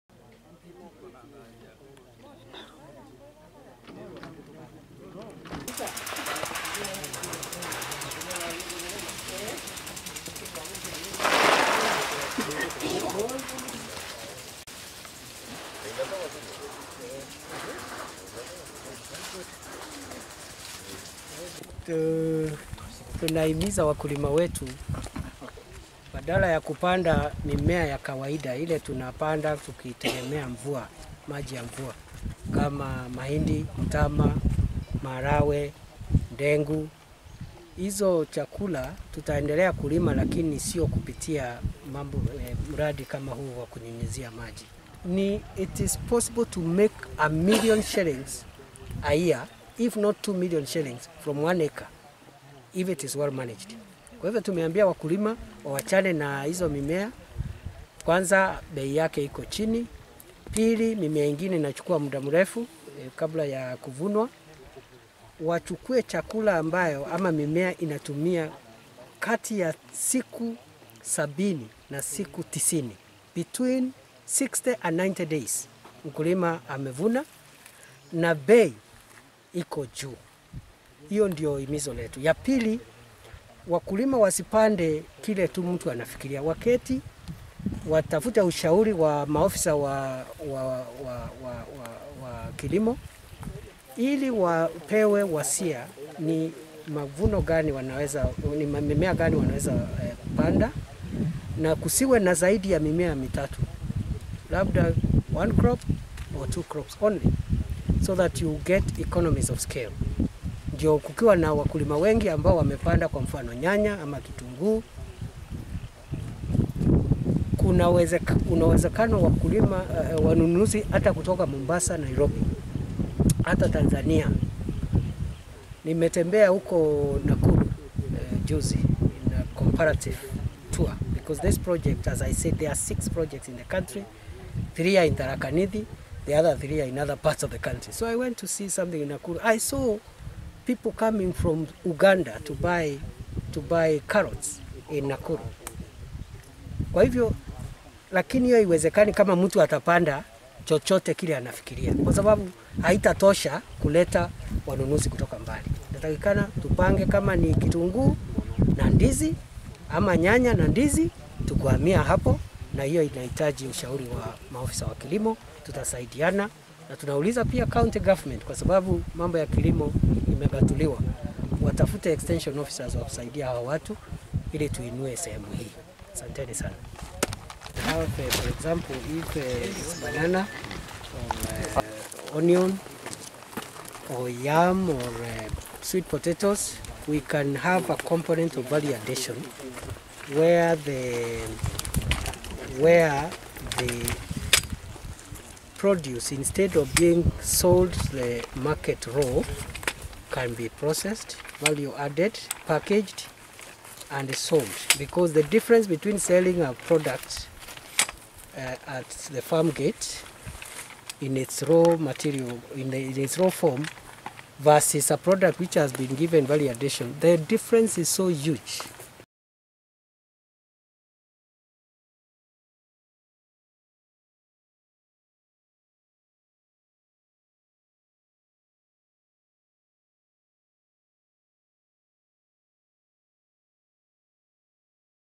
Venture into agribusiness, Kindiki to Igambang'ombe farmers under Makanyanga irrigation project.
Kindiki toured Makanyanga,Rianthiga and Marembo inspecting Small scale, and Value addition program a national government project in Tharaka Nithi county.